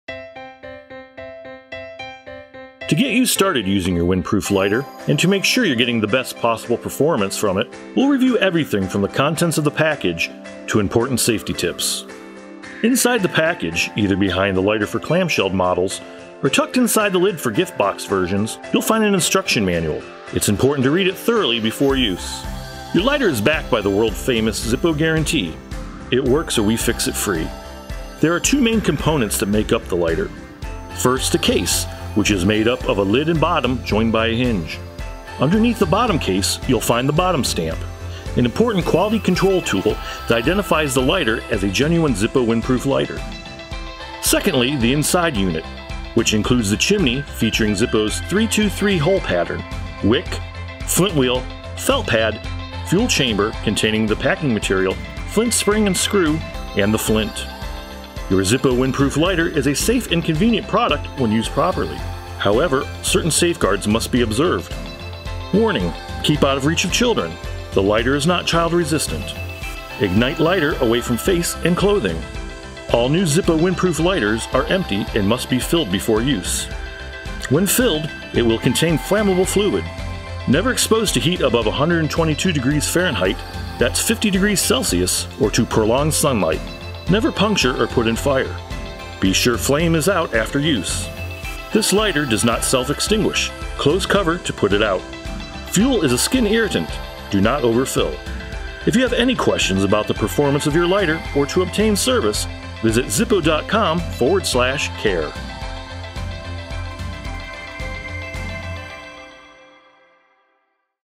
Zippo Instructional: Getting Started
Get started using your Zippo Windproof Lighter. In this video you will learn everything from the contents of the package to important safety tips.